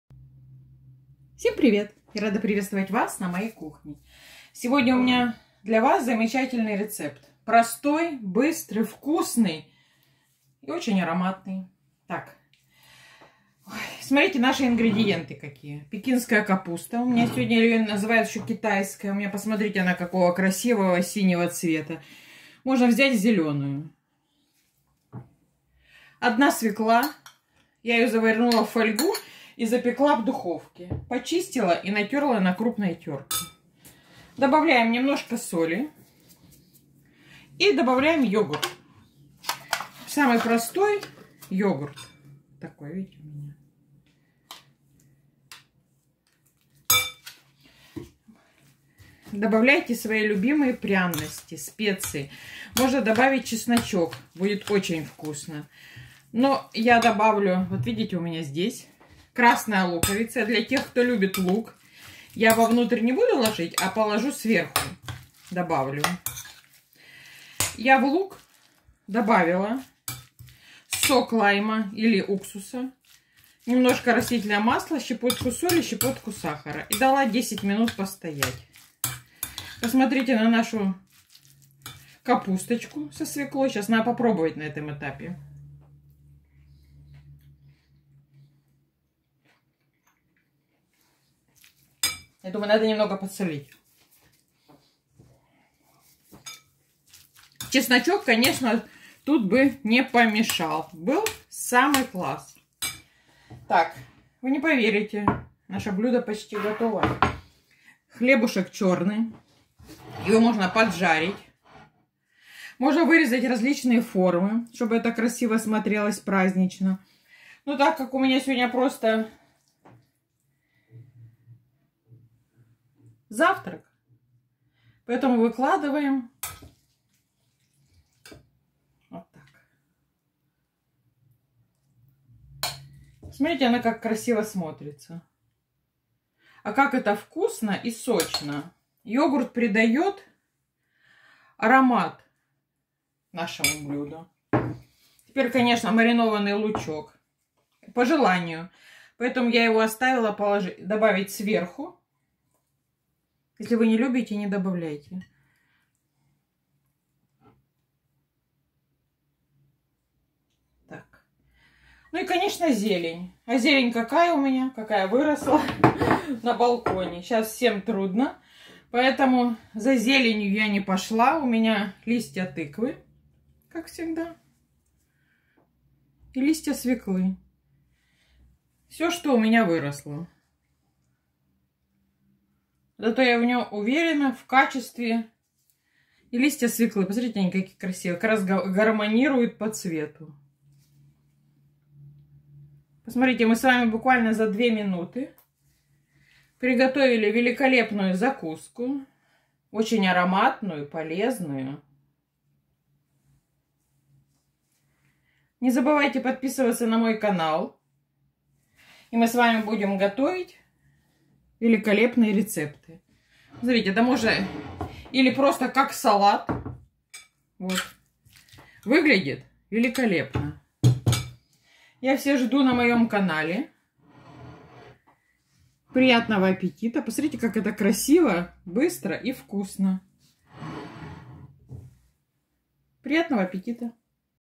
Сегодня необыкновенно простой салат! Легкий, сочный и полезный. Салат с пекинской капустой
Всем привет! Сегодня у нас легкий, простой, полезный салат. Как вам такая порционная подача. Только хлеб надо поджарить.

Просто необыкновенная закуска, из самых простых ингредиентов

Ингредиенты
100 грамм пекинской капусты
1 свекла
50-100 грамм йогурта
Соль
Перец
Красный лук
Ржаной хлеб
Зелень
Последовательность
Свеклу заворачиваем в фольгу. Запекаем до готовности. Натираем на терке
Лук нарезаем полукольцами. Добавляем сок лайма, соль,перец, оливковое масло

Капусту нарезаем, добавляем свеклу, йогурт. По вкусу соль, перец
Хлеб поджариваем. На хлеб выкладываем салат, лук,зелень. Сочно,просто и полезно. Приятного аппетита